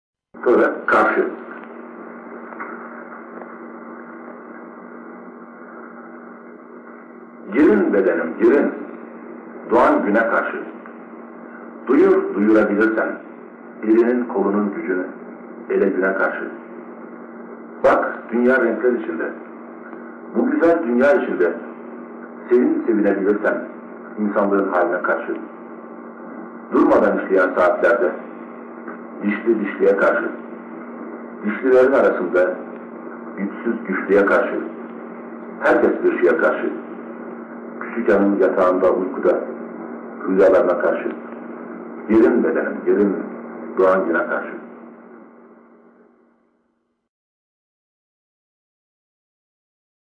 KARŞI - Orhan Veli ( Kendi Sesinden )
Gerin, bedenim, gerin;
Doğan güne karşı.
Duyur duyurabilirsen,
Elinin kolunun gücünü,
Ele güne karşı.

Bak! Dünya renkler içinde!
Bu güzel dünya içinde
Sevin sevinebilirsen,
İnsanlığın haline karşı.

Durmadan işleyen saatlerde
Dişli dişliye karşı;
Dişlilerin arasında,
Güçsüz güçlüye karşı.
Herkes bir şeye karşı.
Küçük hanım, yatağında, uykuda,
Rüyalarına karşı.

Gerin bedenim, gerin,
Doğan güne karşı.